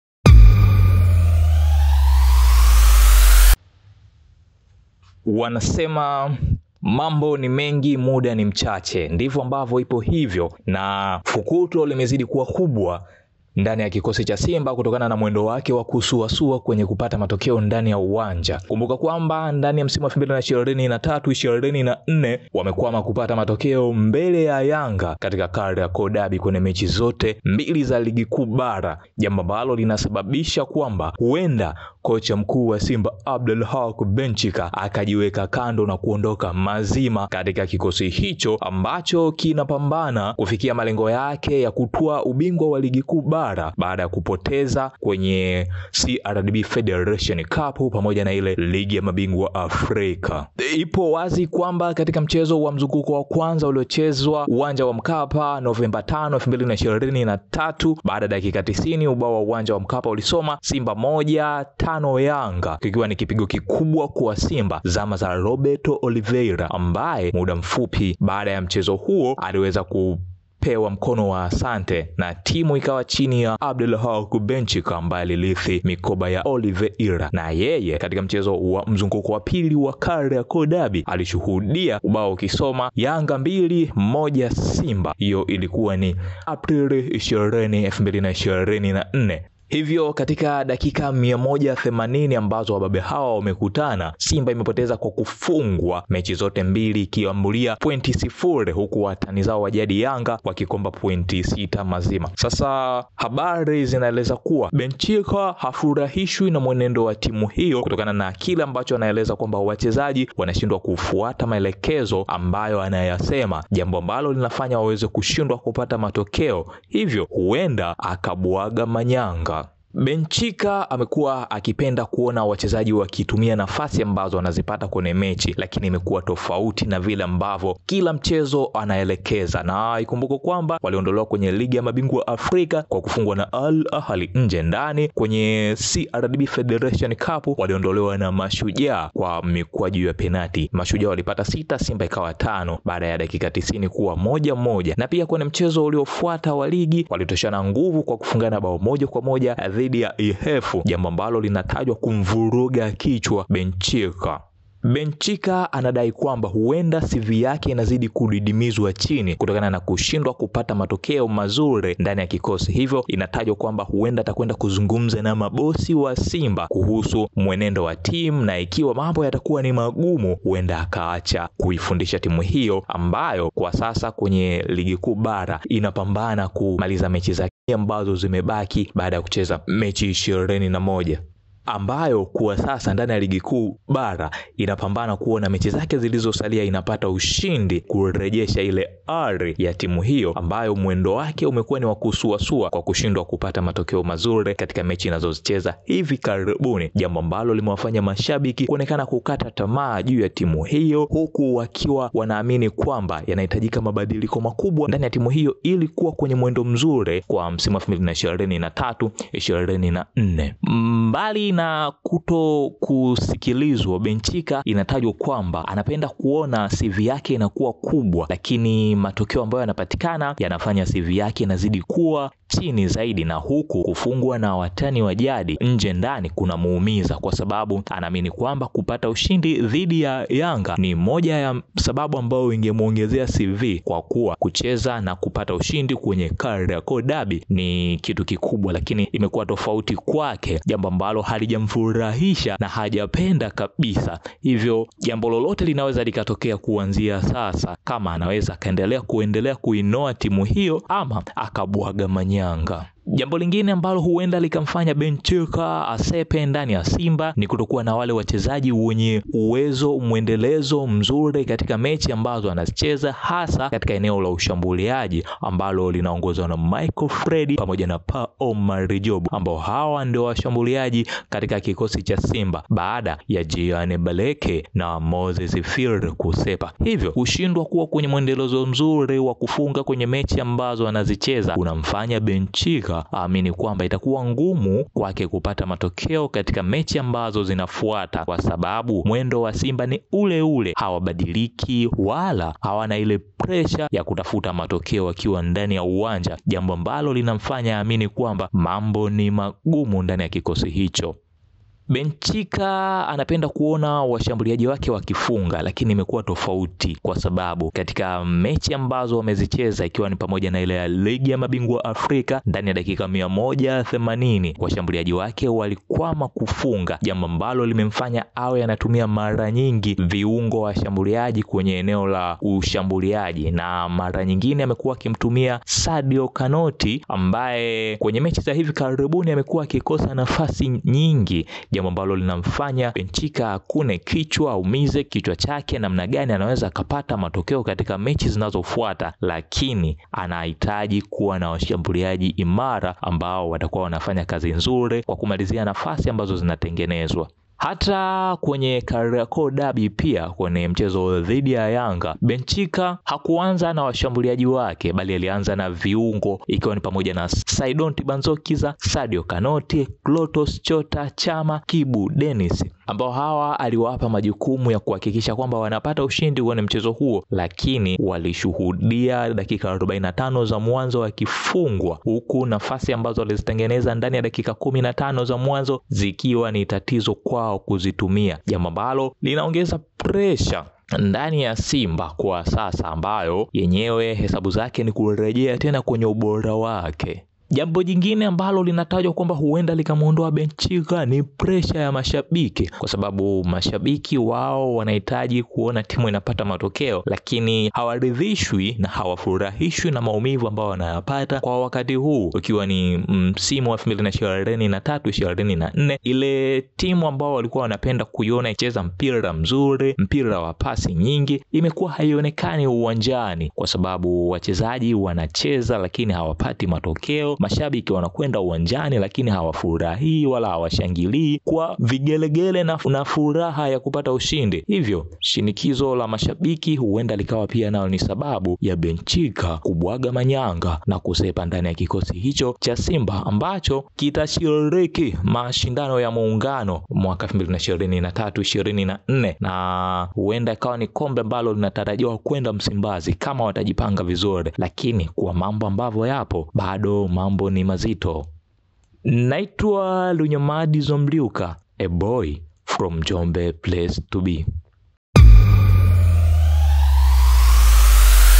ANAONDOKA SIMBA MAZIMA MWAMBA BENCHIKHA SABABU HIZI HAPA
ANAONDOKA ndani ya Simba Kocha Mkuu Benchikha sababu hizi hapa.